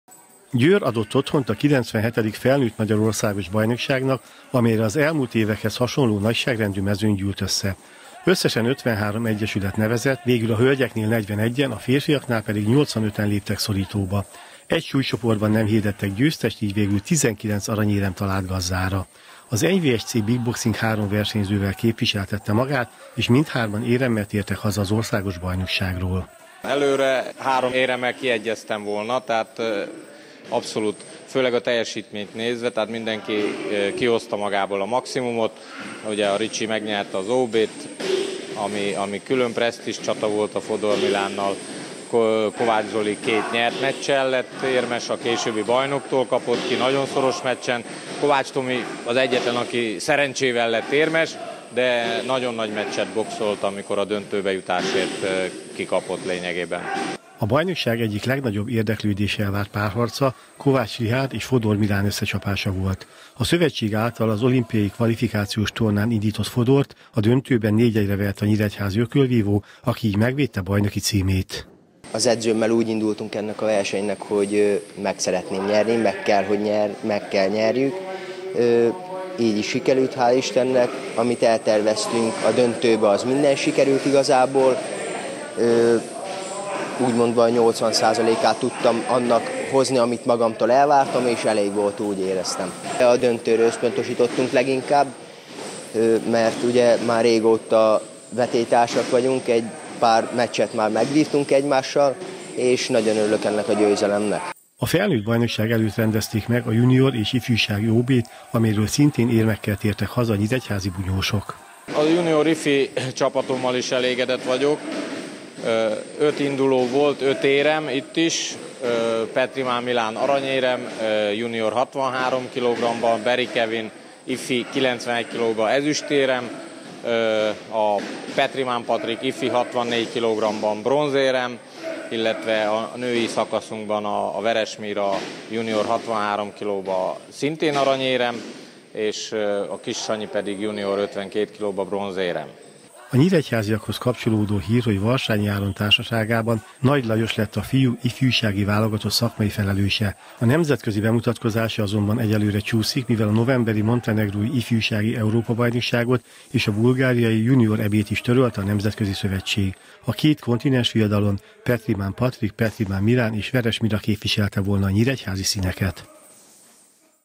Három érem Győrből - Kölcsey Tv
Három éremmel tértek haza a Győrben rendezett felnőtt országos ökölvívó bajnokságról az NYVSC-Big Boxing versenyzői. A 64 kilogrammos súlycsoportban Kovács Richárd megvédte bajnoki címét, a 60 kilogrammos Kovács Tamás és a 69 kilósok között induló Kovács Zoltán pedig a dobogó harmadik fokára állhatott fel.